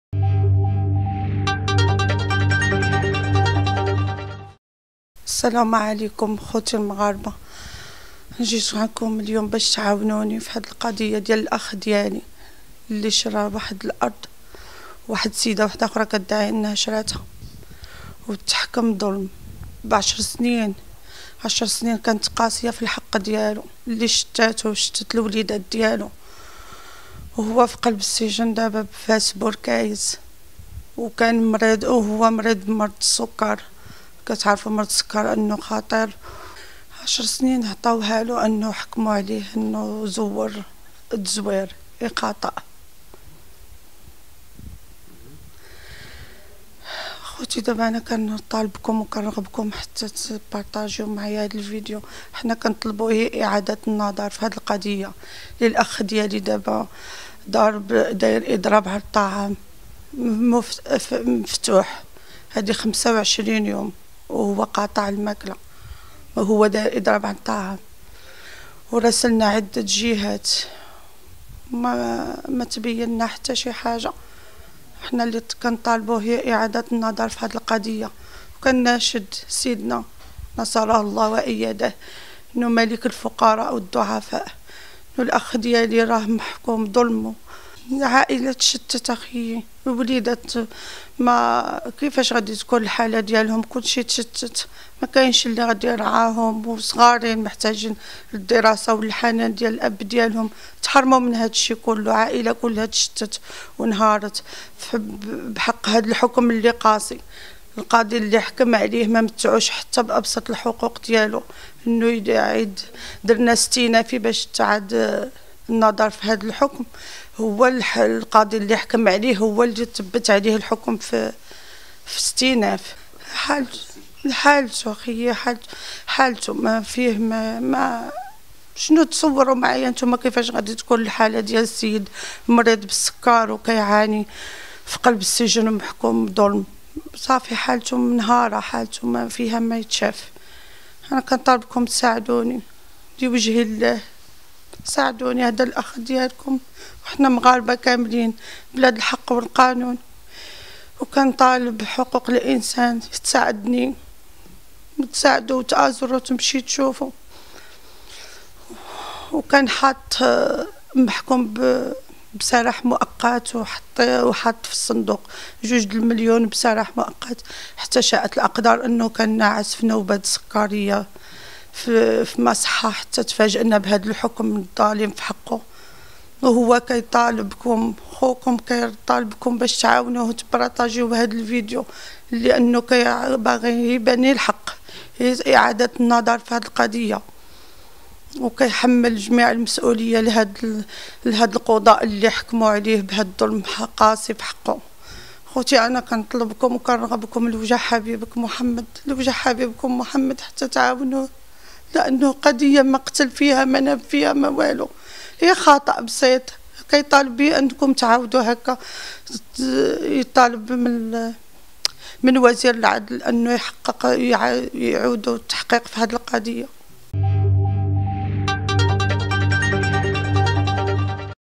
سجين ببوركايز يدخل في إضراب عن الطعام وشقيقته تروي التفاصيل
ناشدت اخت سجين بفاس وزير العدل و الحريات للتدخل في قضية شقيقها بعد اتهامها بالنصب و الاحتيال و تزوير وثائق